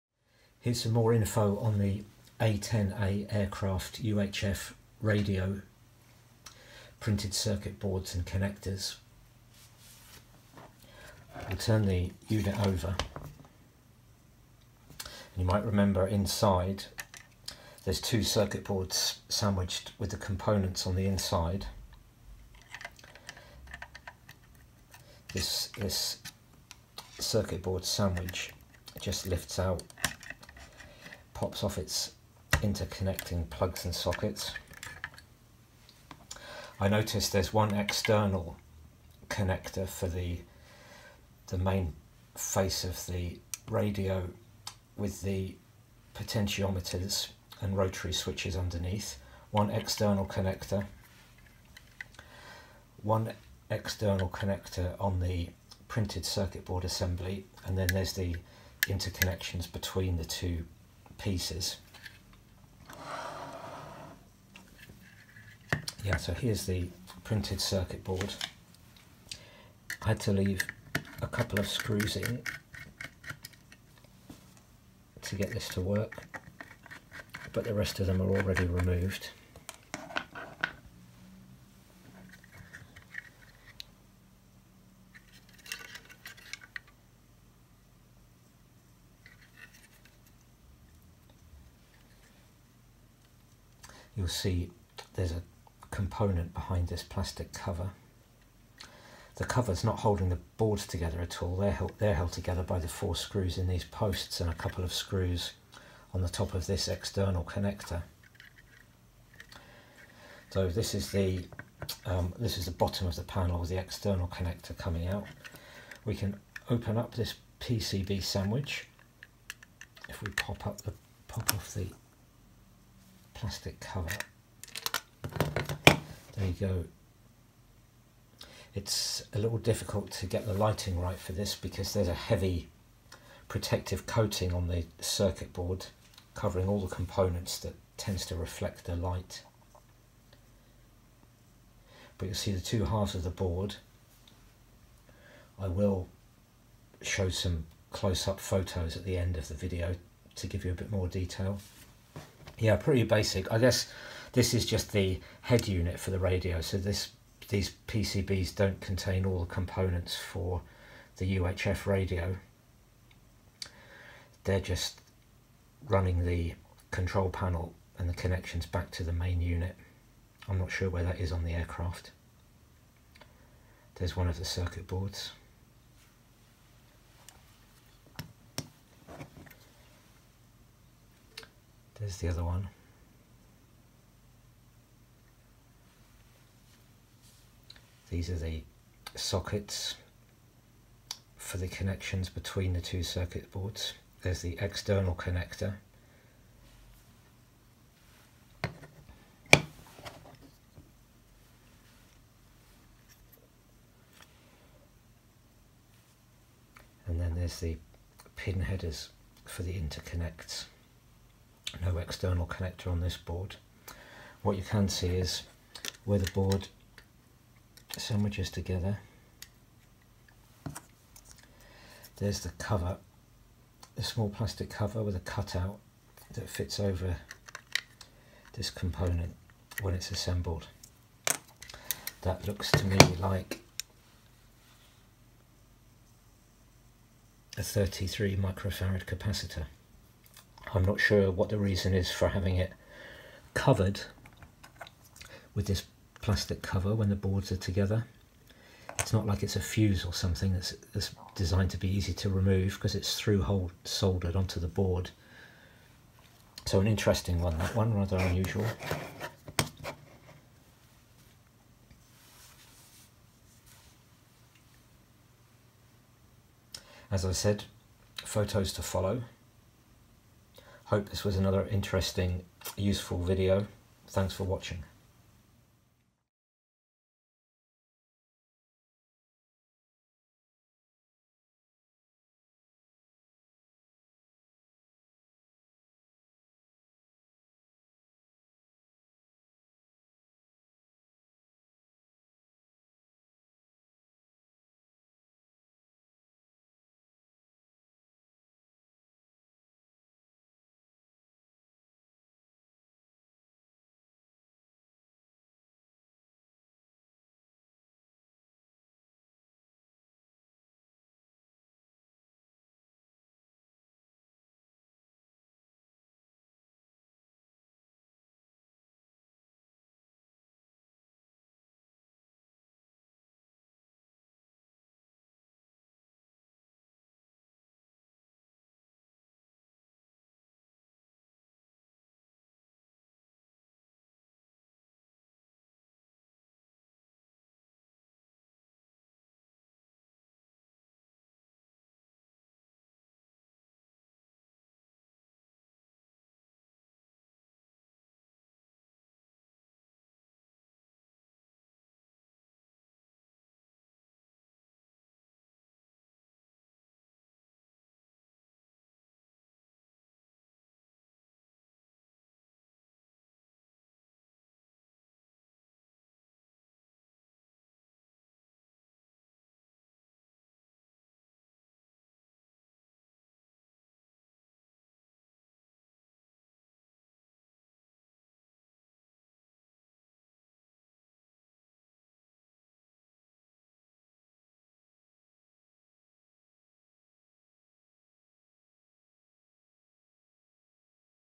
A-10A UHF radio PCBs
A closer look at the printed circuit boards (PCBs) and connectors inside the A-10A UHF radio